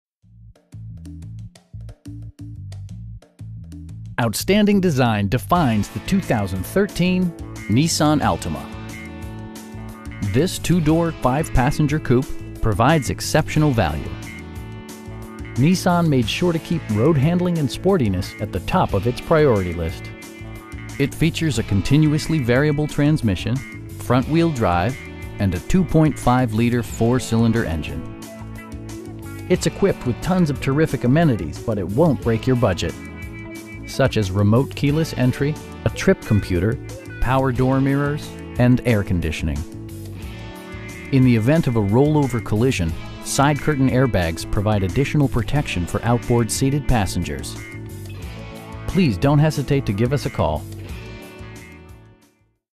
2013 Nissan Altima 2.5 S in Trumann, AR 72472
Central Ford
1801 Highway 69
in Trumann, AR 72472

Outstanding design defines the 2013 Nissan Altima. This 2 door, 5 passenger coupe provides exceptional value! Nissan made sure to keep road-handling and sportiness at the top of it's priority list. It features a continuously variable transmission, front-wheel drive, and a 2.5 liter 4 cylinder engine. It's equipped with tons of terrific amenities, but it won't break your budget. Such as remote keyless entry, a trip computer, tilt steering wheel, power door mirrors, power windows, cruise control, an overhead console, and air conditioning. In the event of a rollover collision, side curtain airbags provide additional protection for outboard seated passengers. We have the vehicle you've been searching for at a price you can afford. Please don't hesitate to give us a call.